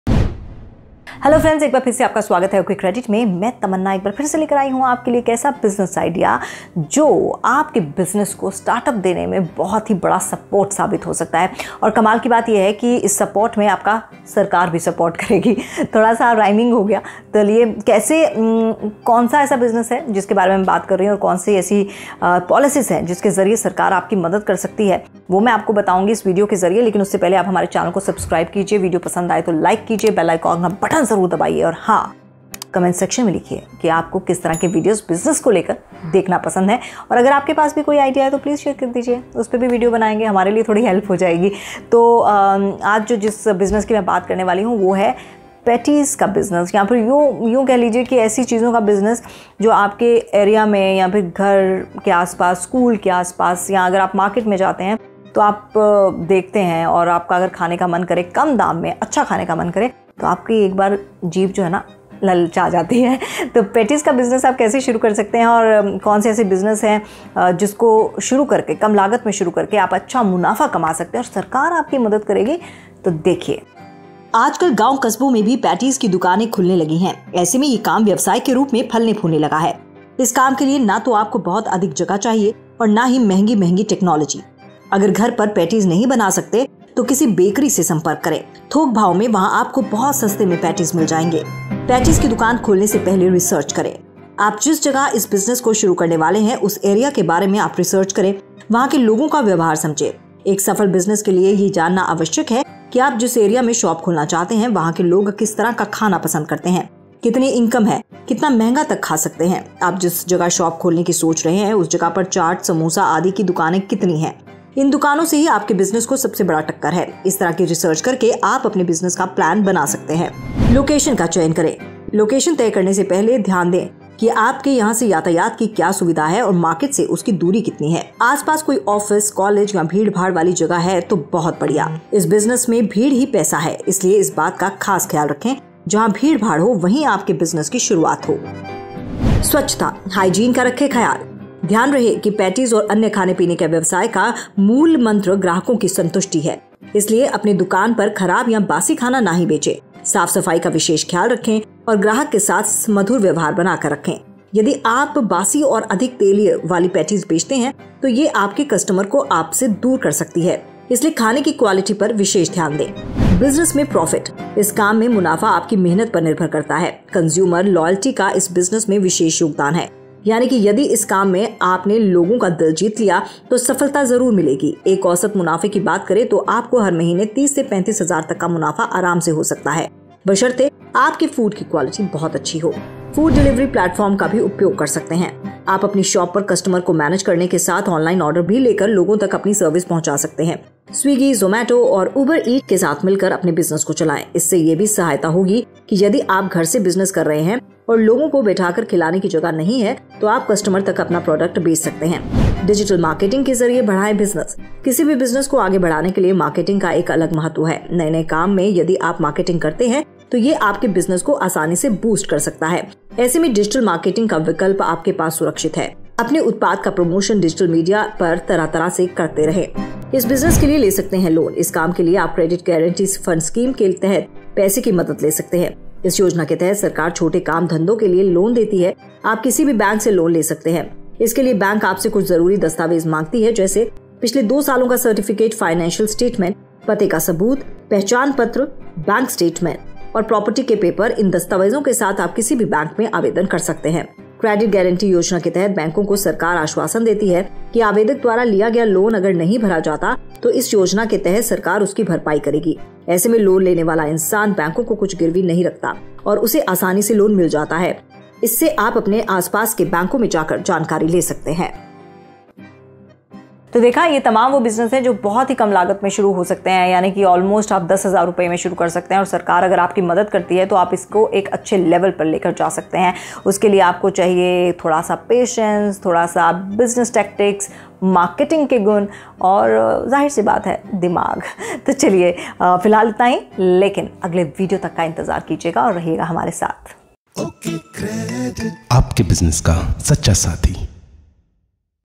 लजीज़ खाने की रेसीपी | Small Business Ideas 2021, Low Investment Business Ideas | OkCredit
बच्चों को लुभाने वाला, शाम की चाय में तड़का लगाने वाला व नाश्ते में स्कूल कैंटीन अन्य जगहों पर हल्का-फुलका कुरकुरा चटकारा लगाने वाला एक ऐसा बिज़नेस जो आपकी कमाई में चटकारा लगा दे।
आलू-मटर से बनी पैटीज का नाश्ते में चलन बहुत पुराना है। हालाँकि आजकल पैटीज कई तरह की आ गई हैं,लोगों के बढ़ते शौक़ को देखते हुए हर प्रकार से बनी पैटीज मार्केट में आ रही हैं।
ऐसे में यदि आप इस बिज़नेस को करते हैं तो यह आपके लिए एक अच्छा विकल्प हो सकता है। इस बिज़नेस की ख़ास बात यह भी है की आप इसे घर से ही शुरू कर सकते हैं। चलिए इस बिज़नेस के बारे में हम आपको विस्तार से बताते हैं।